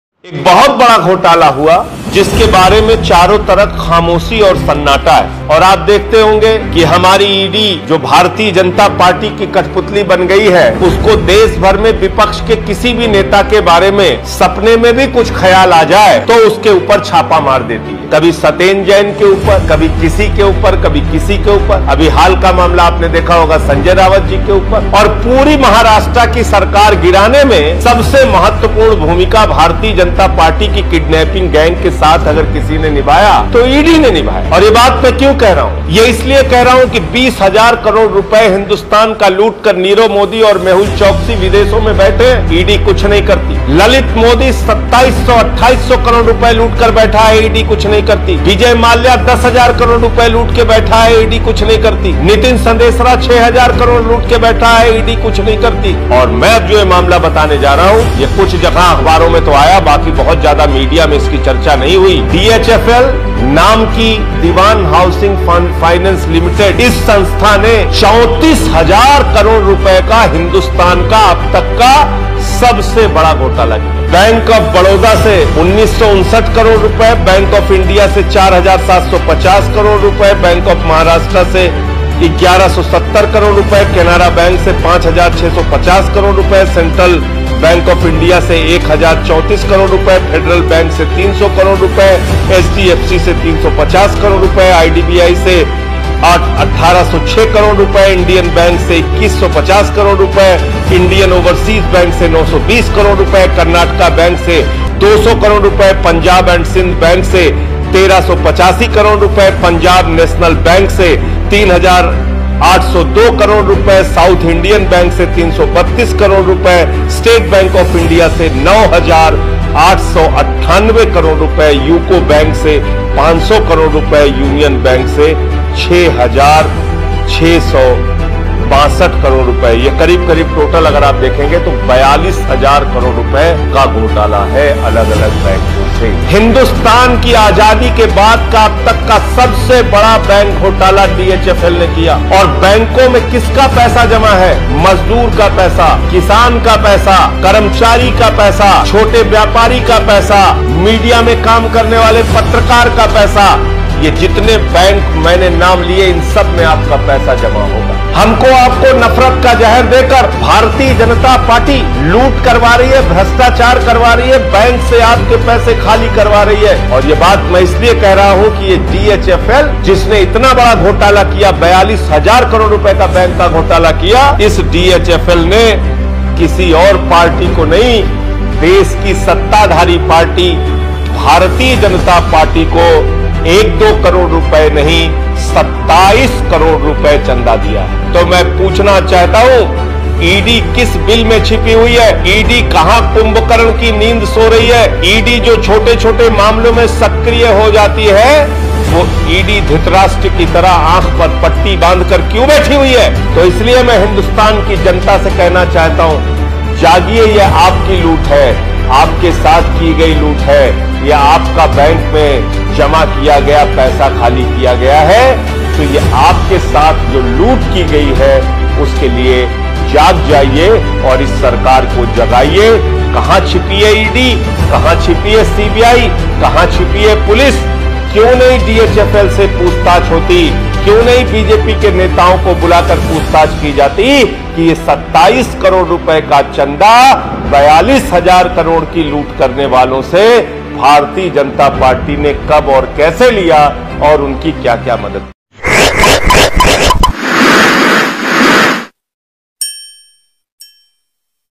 DHFL, which committed the biggest bank scam of India's donated 27 crores to BJP dhfl bjp scam.
DHFL, which committed the biggest bank scam of India's independence, donated 27 crores to BJP, why is ED not investigating DHFL? Why is the ED silent on those who looted the country?

ED has become a puppet of Bharatiya Janata Party. Sanjay Singh